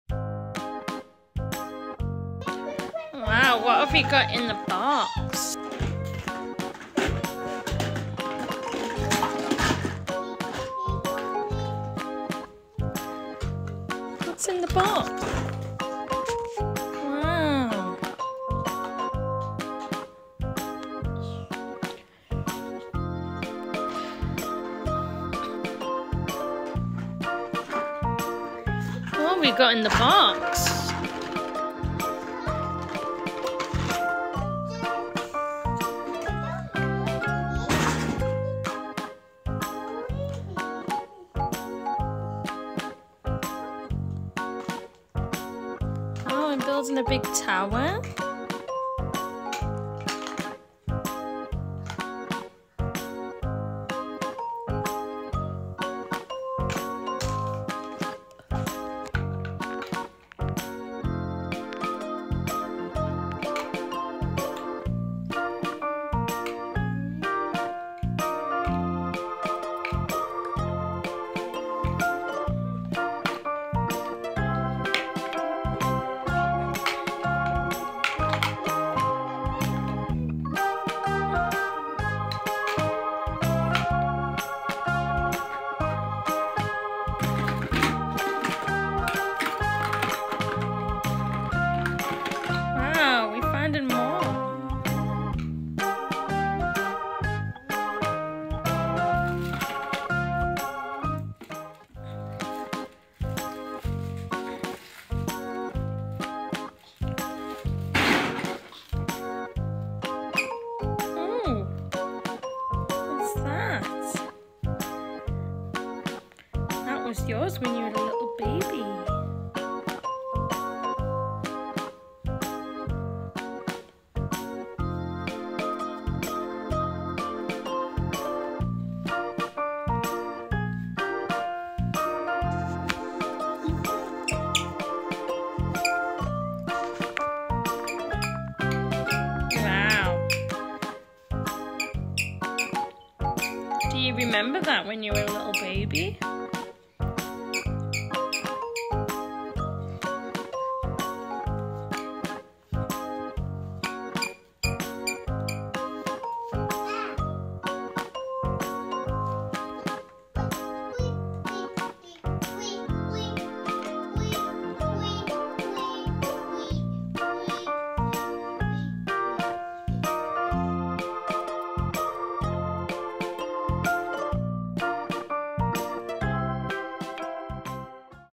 Kid Playing With Blocks Building + Exploring More Toys
Jordan playing with blocks and building towers and exploring more toys.